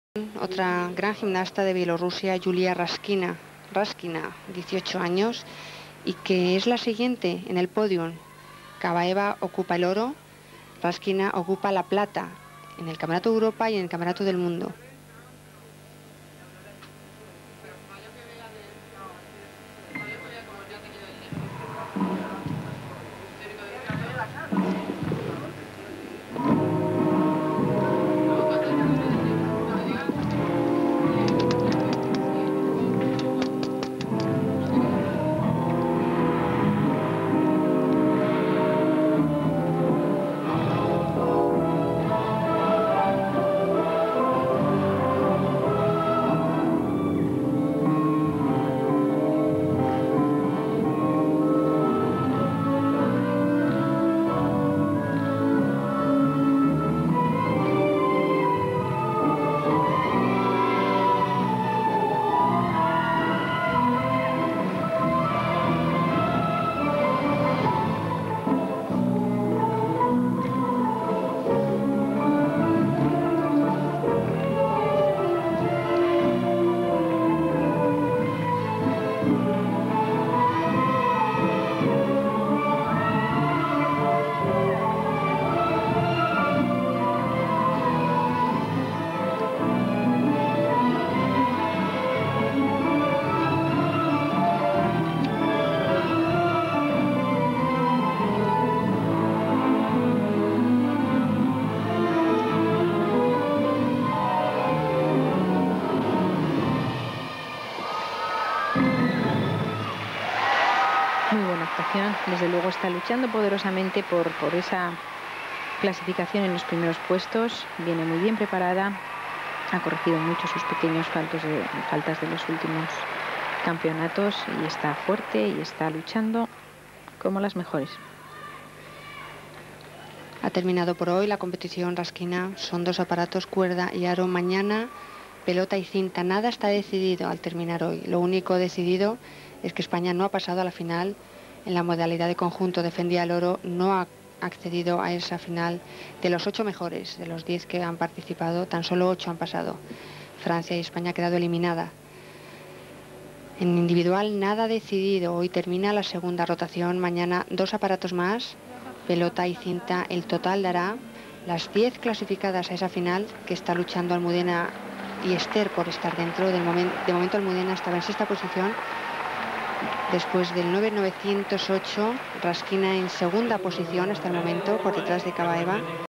Yulia RASKINA (BLR) hoop - 2000 Sydney Olympics qualifs
Yulia RASKINA (BLR)

'00 Olympic Games in Sydney - Qualifications

9.908
Qualifs rank : 2nd